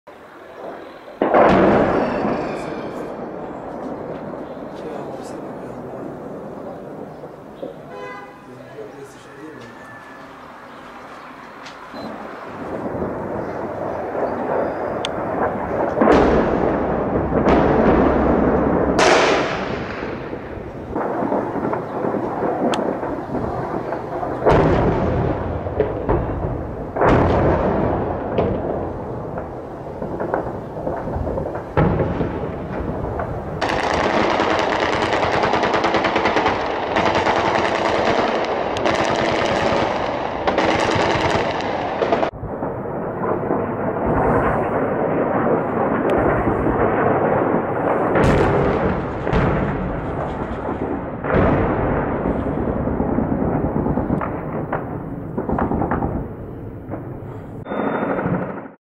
Ataque sobre la ciudad de Daraa por la aviación rusa contra los rebeldes al Gobierno de Assad
Aviones rusos han bombardeado estos días la ciudad de Daraa al suroeste del país (cerca de la frontera con Jordania) para acabar con la resistencia al gobierno de Assad que ejercían cientos de rebeldes en los barrios del norte.